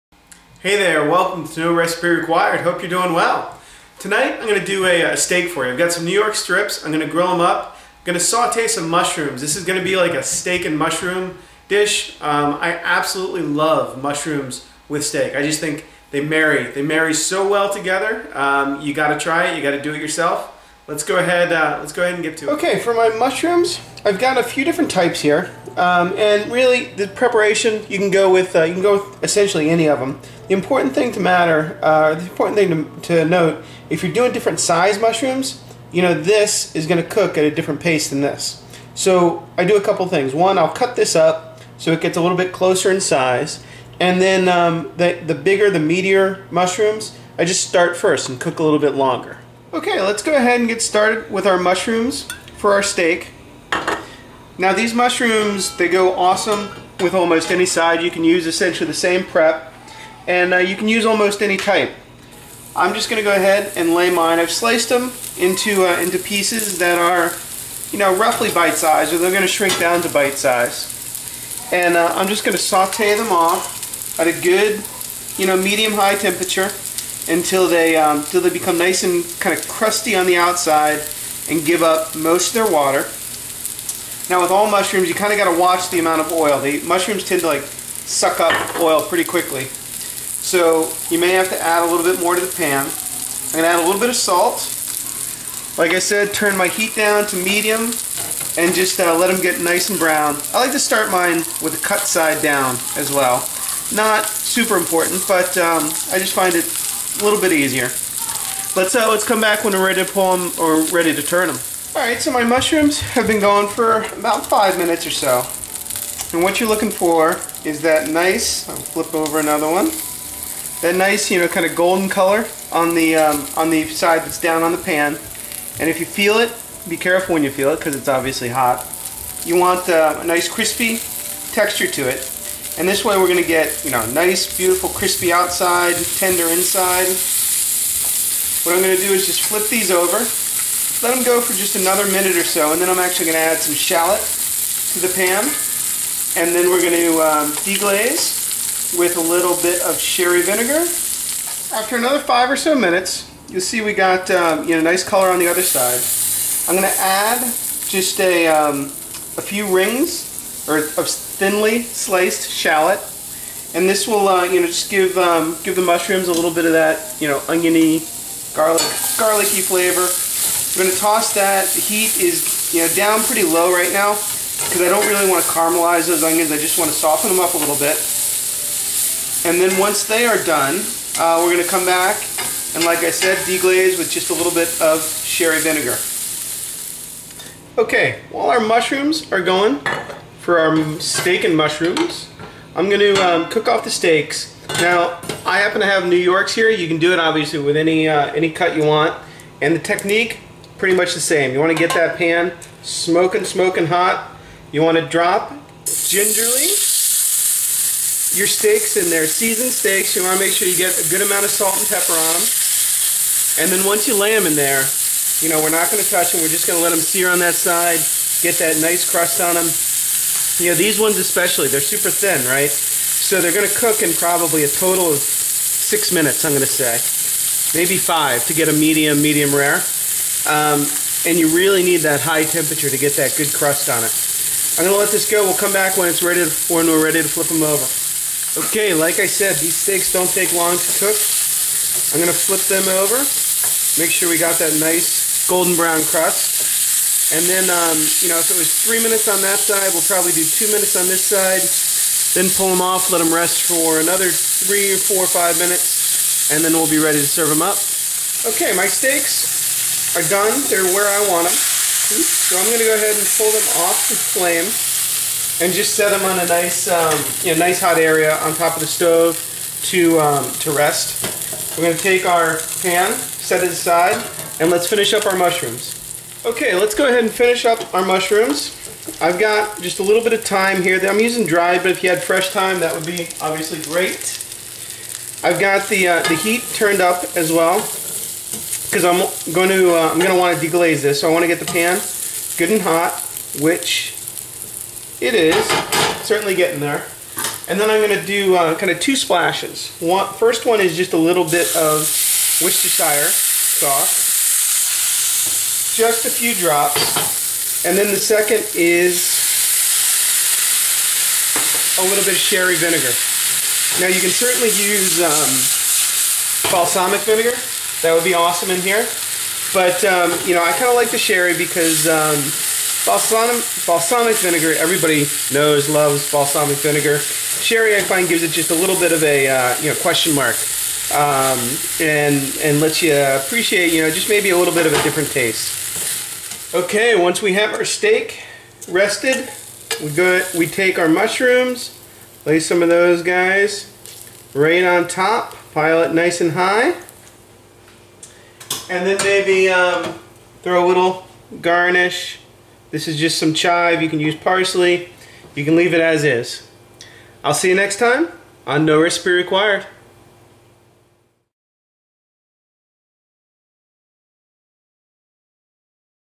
New York Strip Steak with Mushrooms - NoRecipeRequired.com
I love New York STrip Steaks, and Steak and Mushrooms are a perfect combination...assuming of course they are both perfectly cook.  In this recipe video, I pan roast some New York Strip Strip steaks on the stove top, until they are perfectly seared on both sides, and nice and medium rare in the middle.

To serve with those New York Steaks, I'll also show you how to saute off some Mushrooms (in this case King Trumpet, but use any you'd like) which are a perfect match.  In fact, if just deglaze the pan with a bit of wine, stock or butter, you'll have yourself a nice little steak sauce.